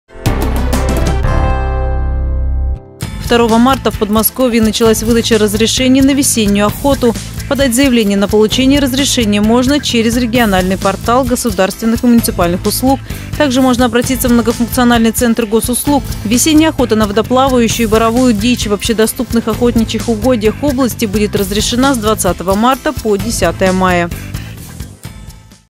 Весенняя охота
2 марта в Подмосковье началась выдача разрешений на весеннюю охоту. Подать заявление на получение разрешения можно через региональный портал государственных и муниципальных услуг. Также можно обратиться в многофункциональный центр госуслуг.